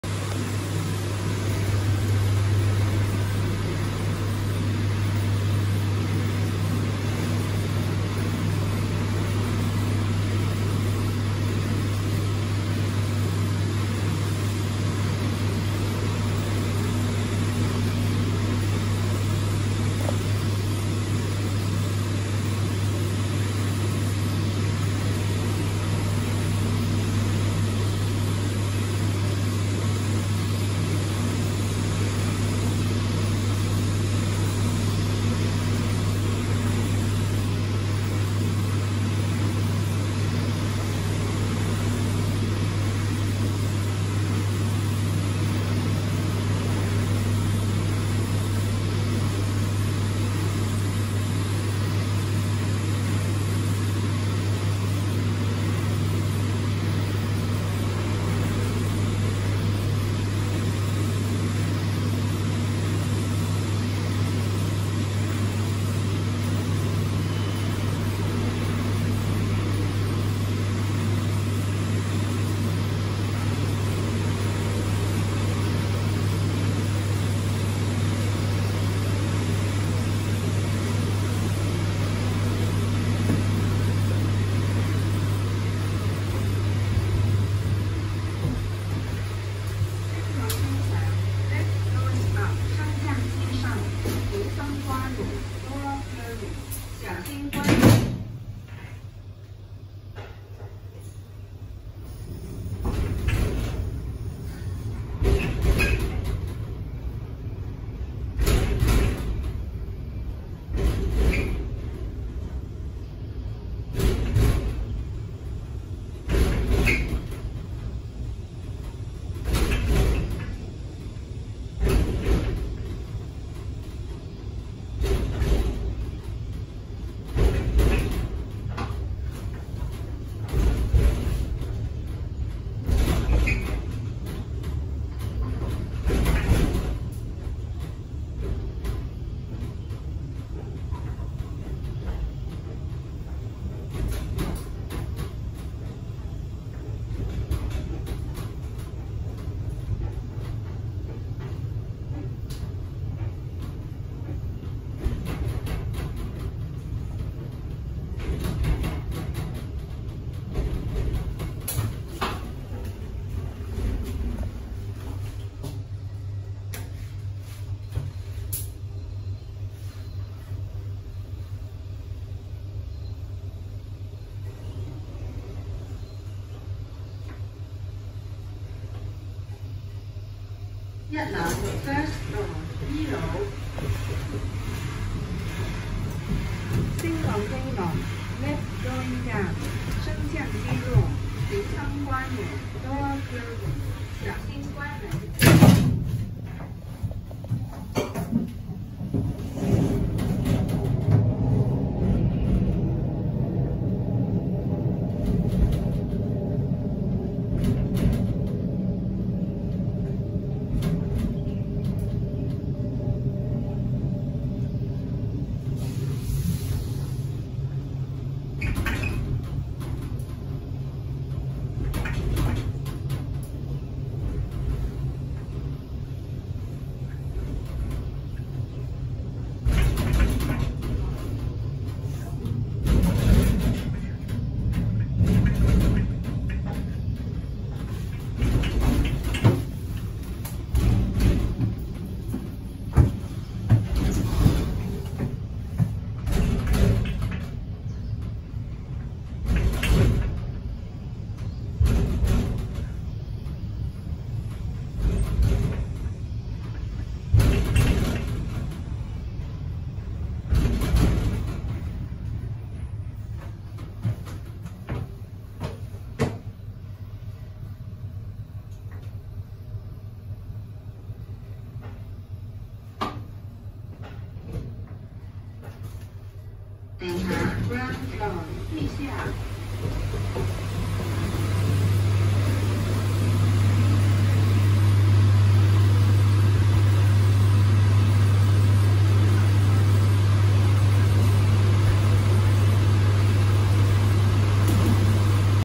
【強烈季候風信號生效】沙田寶福山（原有部分）Schindler無機房斜道升降機（１號升降機）
寶福山（Po Fook Hill）是香港一個骨灰龕場，位於香港新界沙田排頭街（沙田政府合署後方），主要服務為提供私立的骨灰龕位及殮葬服務。由香港殯儀業人士馮成創立，1991年4月26日進行開光禮。

根據1990年無綫電視播出的《星期一檔案》的報道，寶福山原發展商的大股東是中國海外。

寶福山佔地200,000平方呎，採用現代化企業管理，有營業部及顧客服務部。寶福山亦設有直達山頂的登山扶手電梯、斜行升降機及升降機，主要提供骨灰龕和神主牌兩種供奉形式，亦根據靈位的風水位、高低、行數等多方面設定價錢，由數千元至數十萬元不等。另外，寶福山提供24小時管理服務，並定時為先人上香和清潔石碑，在香港的墓園而言亦屬罕見。

地址：香港新界沙田排頭街12號寶福山（原有部分）

再次投入服務年份：2019年

本機停靠樓層：G, 1（２層）

本機為前往墓地主要區域的斜道升降機，取代自1990年投入服務的Schindler/Gangloff斜道升降機。

2019年，本機被更換為新機，客卡和機件都被換上新的，載客量增加至12人，並加裝了Tonic樓層廣播。在更換工程當中，大堂同時也被翻新。設備屬通用件，按鈕採用了Dewhurst Jumbo US90-30款式，而樓層顯示器為Tonic DM點陣款式，設計與普通的升降機無異。更換後仍設有座位，然而改用了摺疊式。

Year of commission after the renovation: 2019

This lift serves: G, 1 (2 floors)

Schindler M.R.L. Inclined Traction Elevator, Po Fook Hill (original part), 12 Pai Tau Street, Sha Tin, N.T., Hong Kong